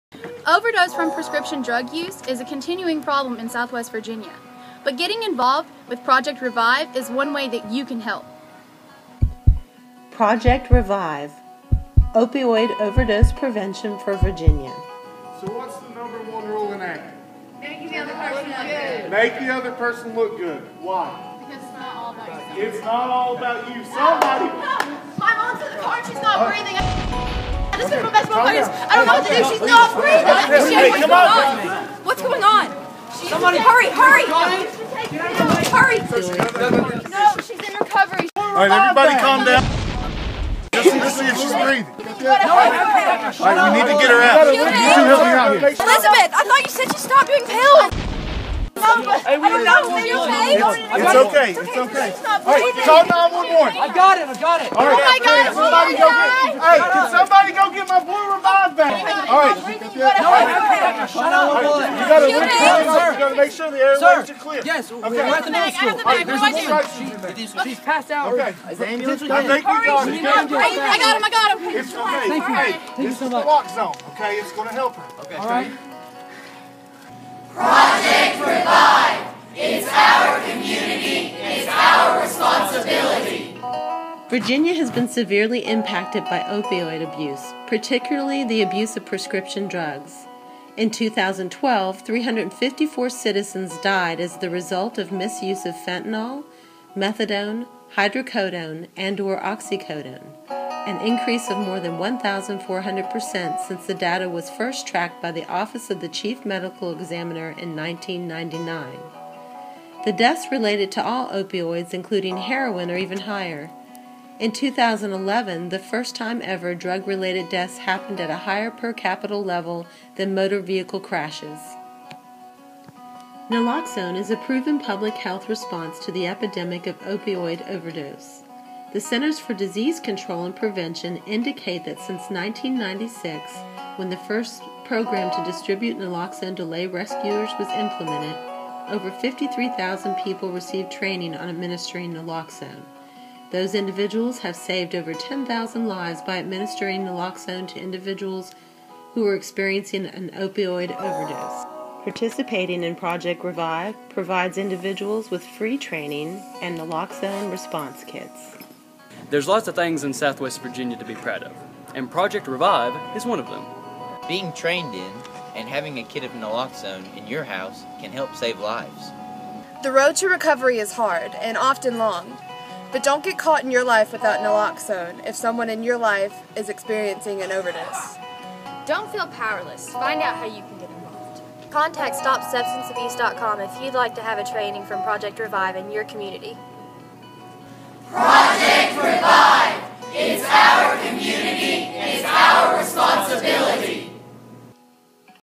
Project REVIVE in Southwest Virginia for the use of Naloxone to prevent opioid overdose.
Project REVIVE is a pilot project of the Commonwealth of Virginia which makes Naloxone (Narcon) available to lay rescuers to reverse opioid overdoses. For information on receiving a FREE training and rescue kit in Southwest Virginia, contact StopSubstanceAbuse.com. It's Our Community. It's Our Responsibility.